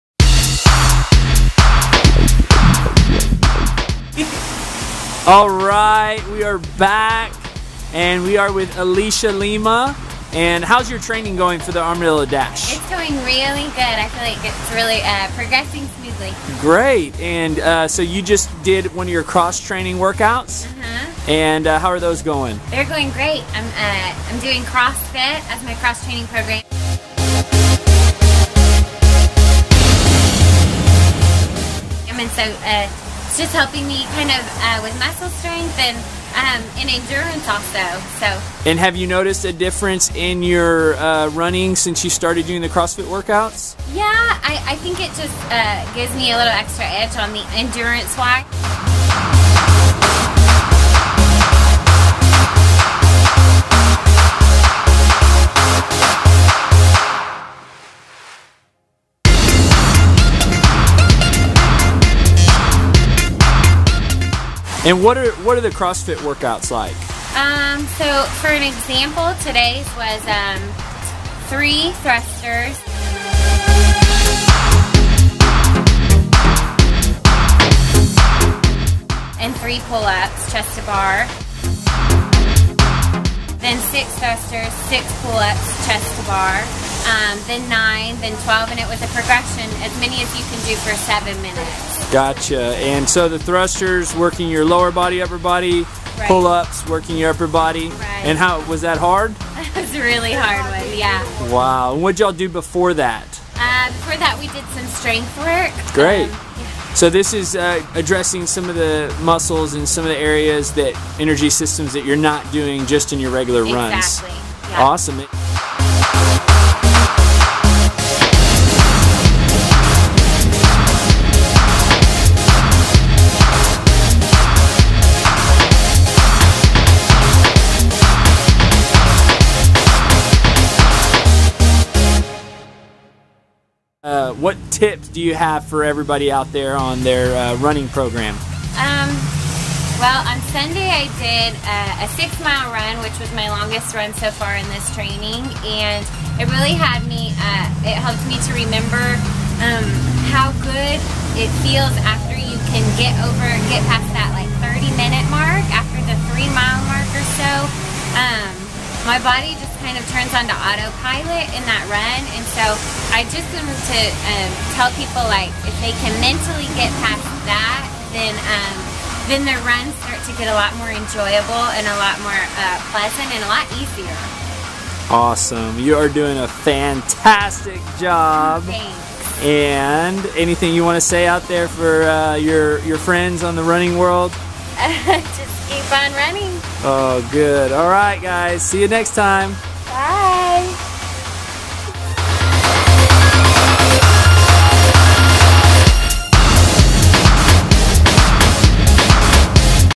2013 Armadillo Dash Training Video #4: week 10
BoomFit is providing a 16 week training program leading up to the Armadillo Dash - March 3rd, 2013 in College Station, TX. We we will have weekly posts and videos. This video features Alicia Lima, who just had a baby on August 27th, 2012 and is training for the Armadillo Dash.